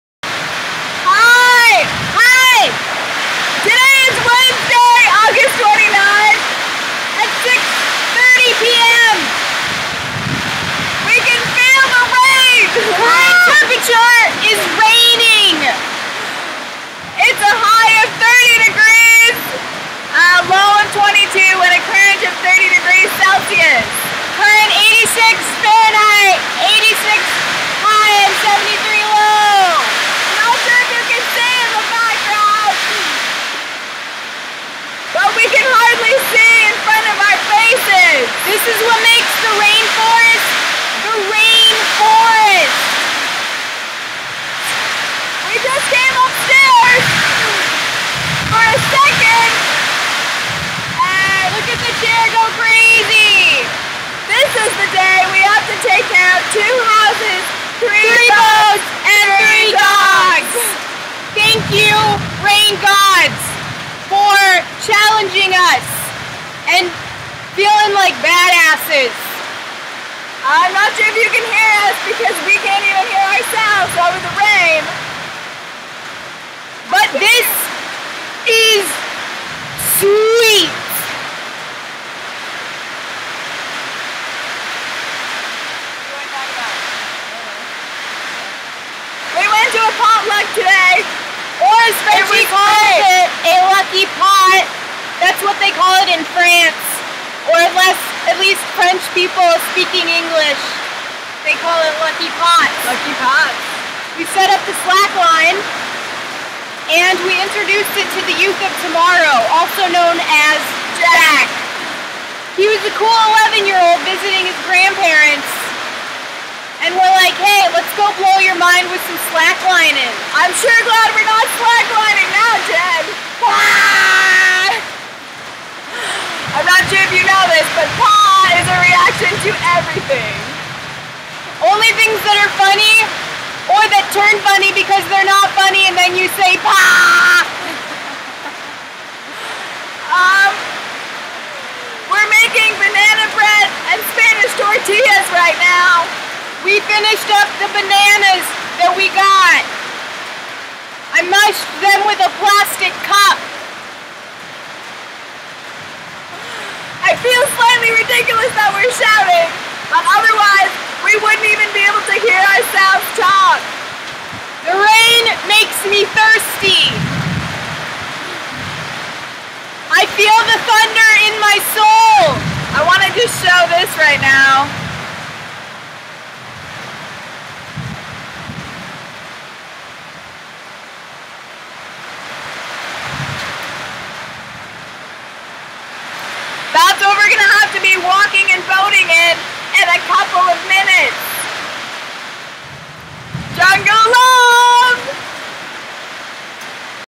#45 Panama Log 2012-08-29
Day 45 - It rained 2.5 inches in just over an hour! We're shouting through the storm...